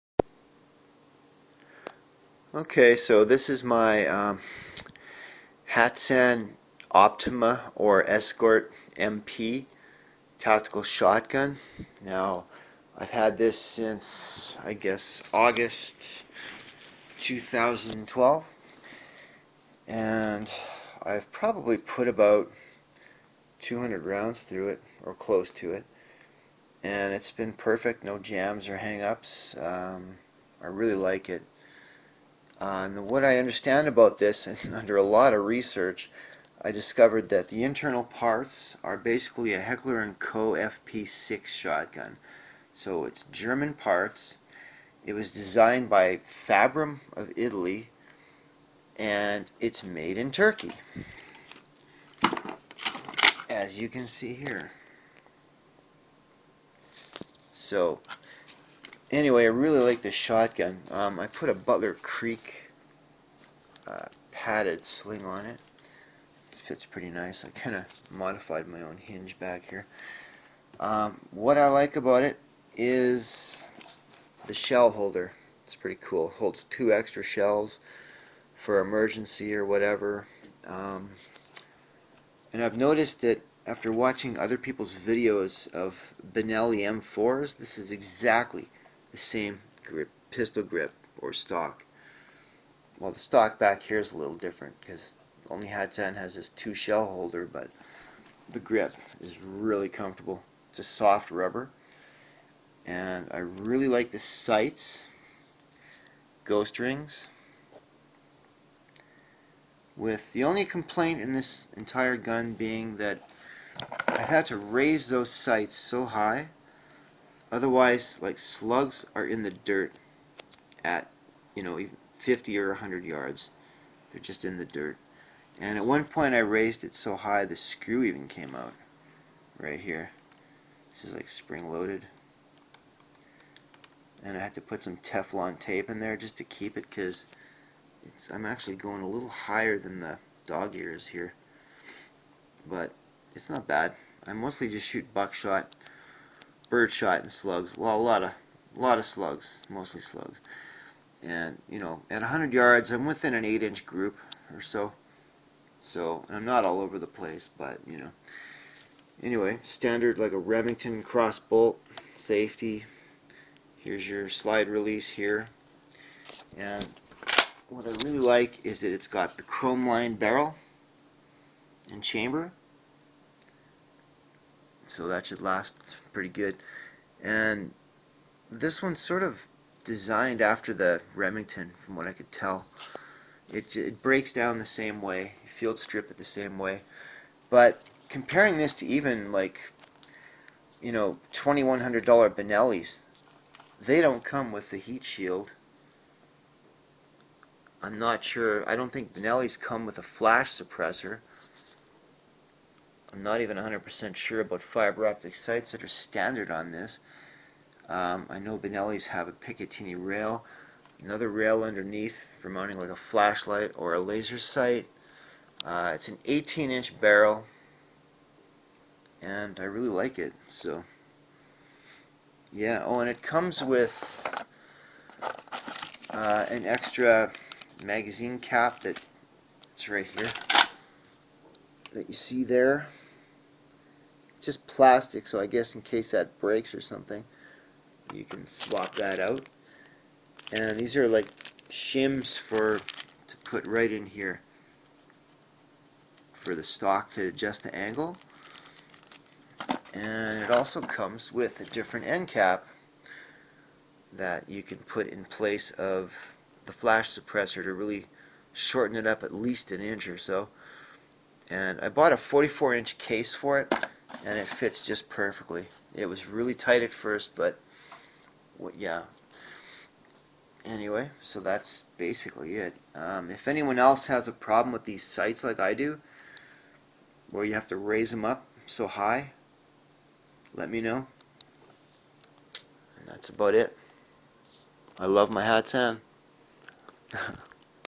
Hatsan Escort MP Shotgun Review
my 12ga Shotgun review.  This shotgun retails for $399 in Canada and has more features than a $2100 Benelli M4!! Pump action of course and is also called the "Optima" in Canada. I paid only $360.  If anyone knows which mag-tube extender can be used post it. I'm thinking one for Remington 870's.  IF ANYONE KNOWS exactly how to remove the magazine spring retainer to clean the Mag tube for cleaning, let me know PLEASE